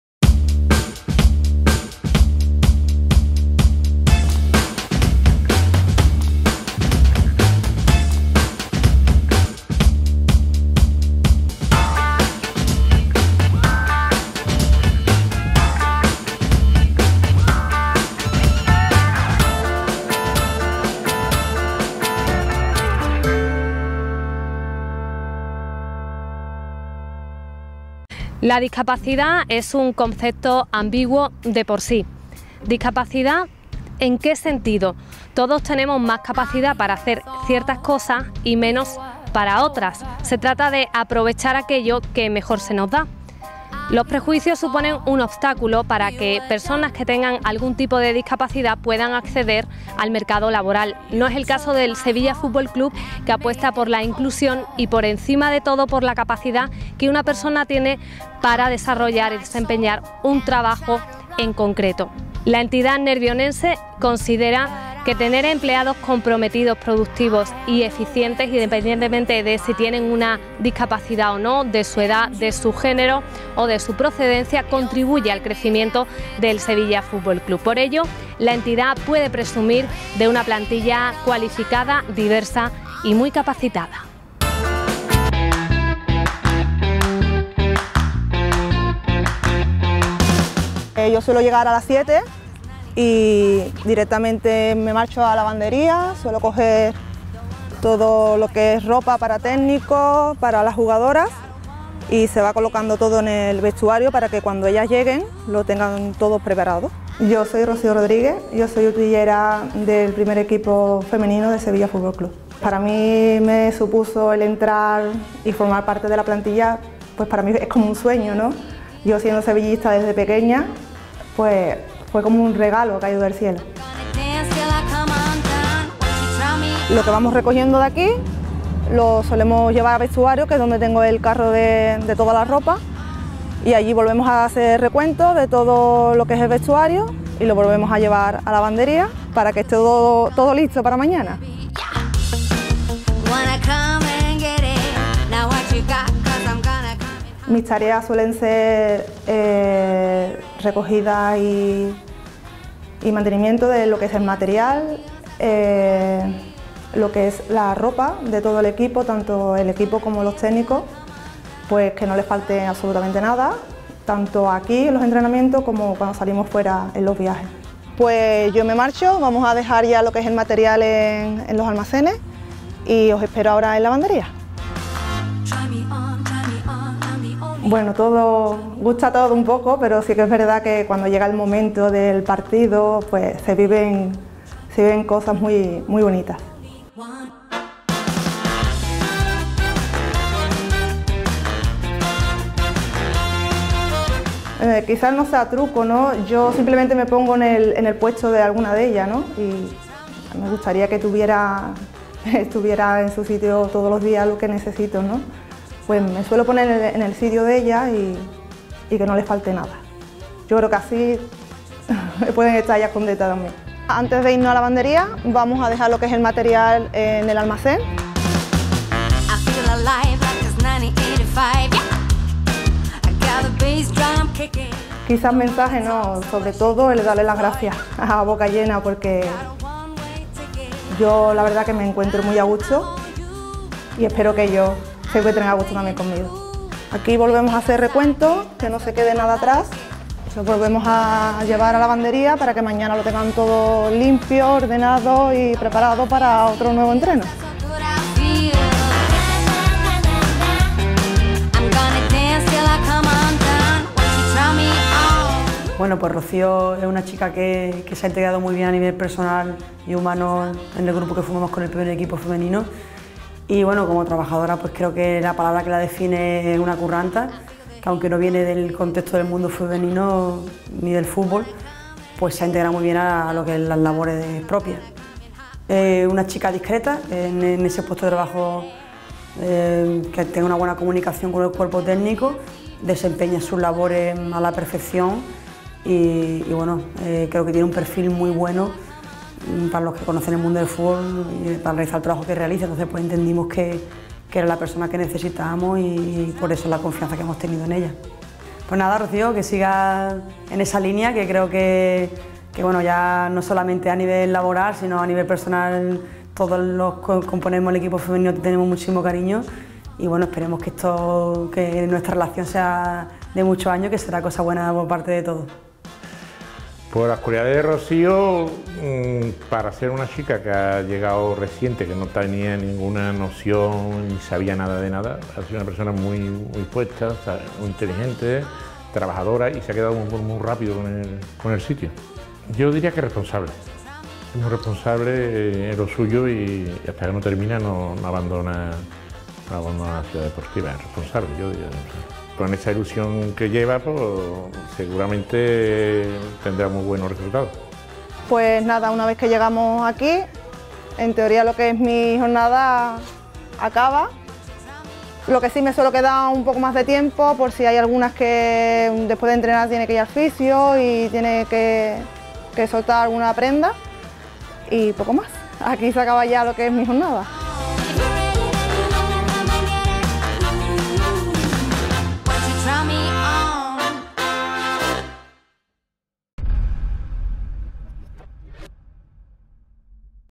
CapacitadosSFC: Capítulo 6: Rocío Rodríguez, una curranta nata entregada al Sevilla FC Femenino
CapacitadosSFC: La responsabilidad y profesionalidad de la utillera del Sevilla FC Femenino.
Ya lo sabes, nunca nos rendimos. Te contamos una nueva historia en CapacitadosSFC.
Capítulo 6: Rocío Rodríguez, una curranta nata entregada al Sevilla FC Femenino.